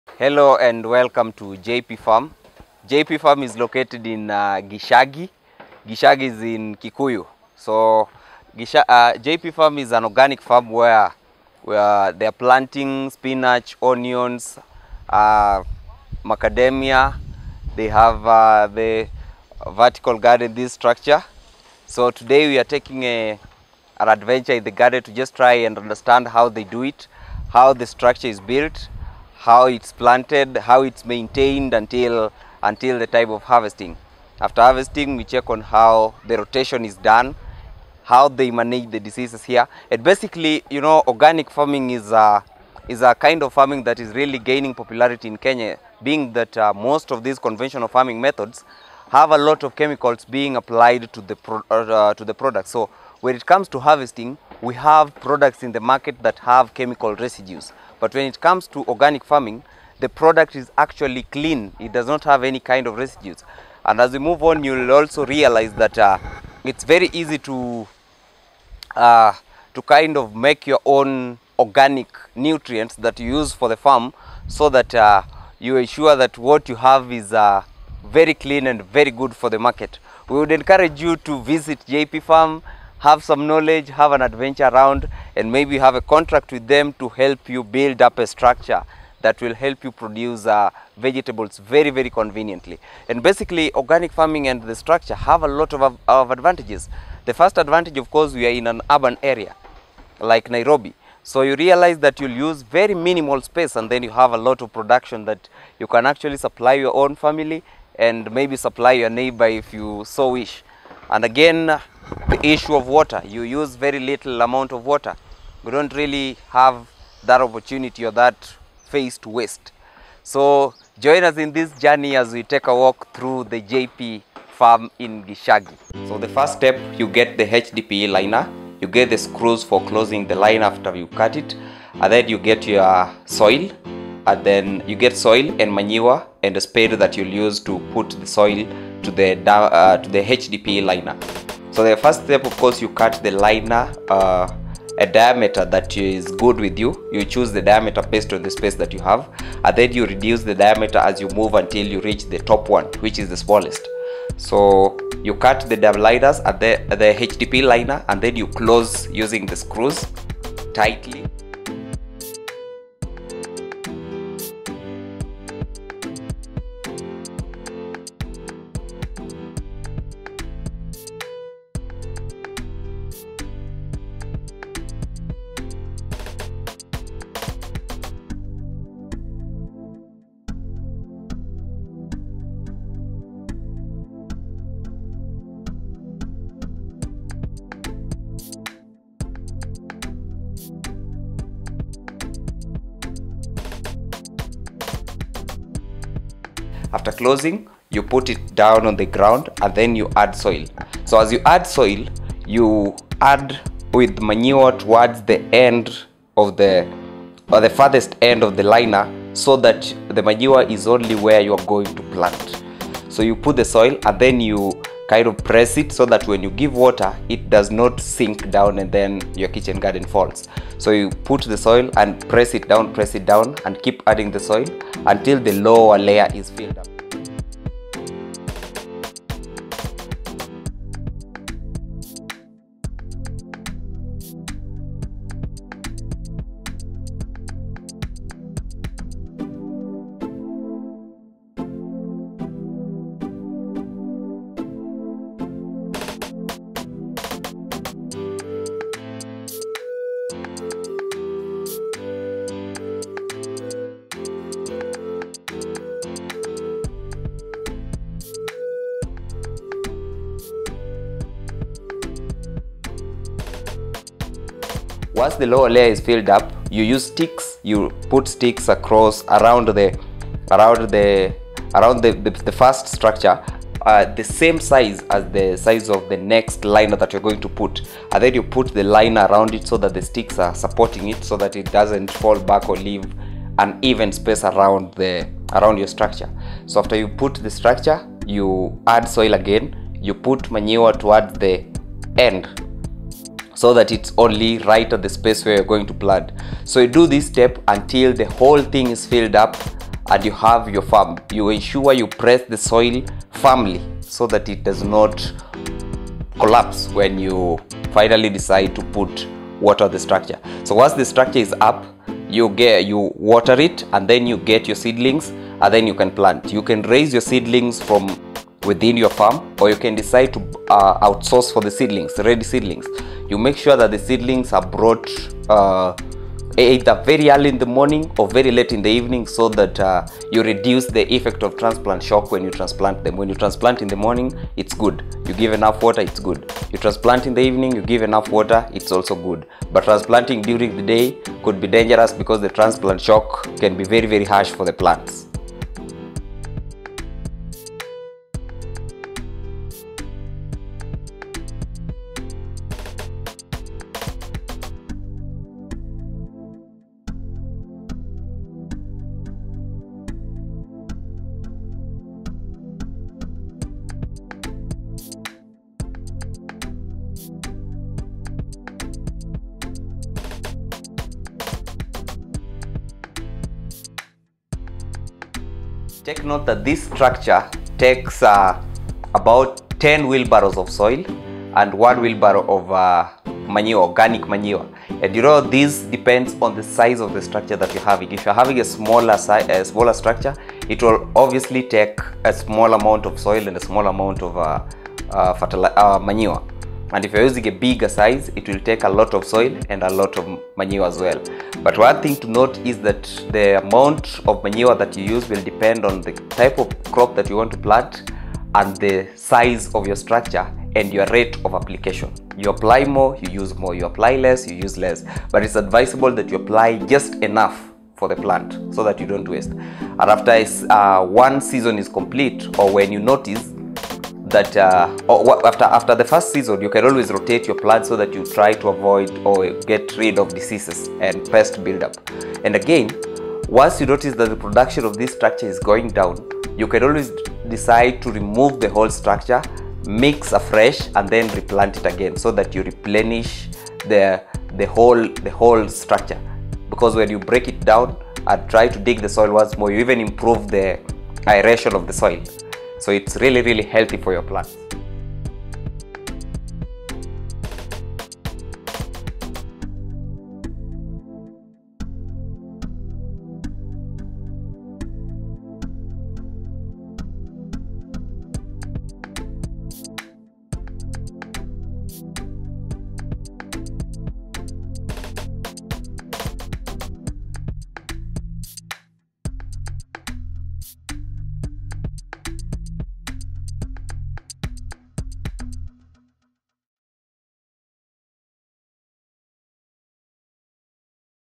Amazing Agriculture Cone Garden / Multi Storey Garden Structure Setup
A cone garden/multi storey garden is a vertical structure used for vegetable production often in urban areas where space is limited. The help in utilization of production factors and maximization of output.

Video shot by BlackArt Mansions LTD